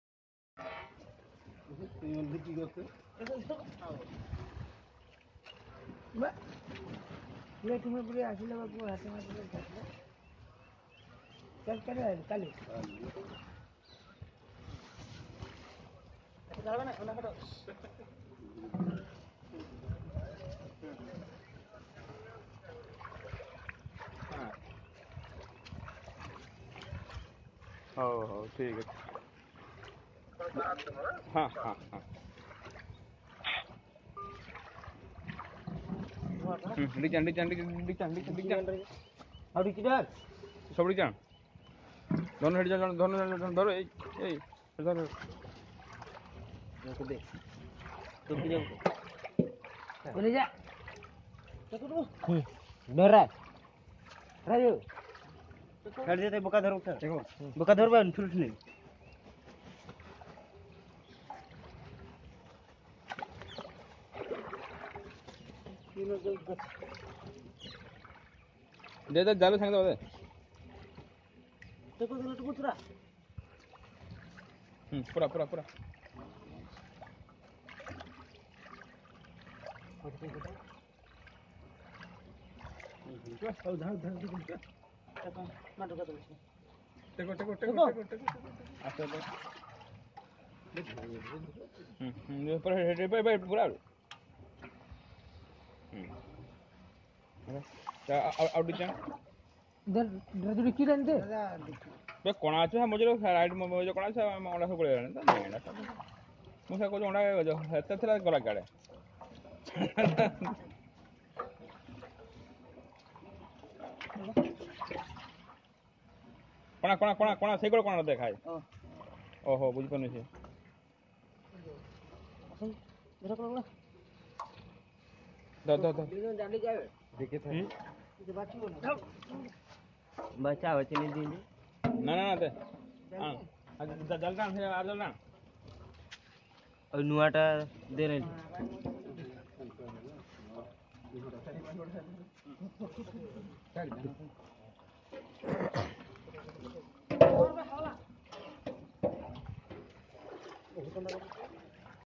Eggs of Indian grass carp done by Kailash Hatchery, odissa
Harvesting the eggs of grass carp with zero addition of chemicals or steroids to the brooders. First time in India with more than 40lakh eggs.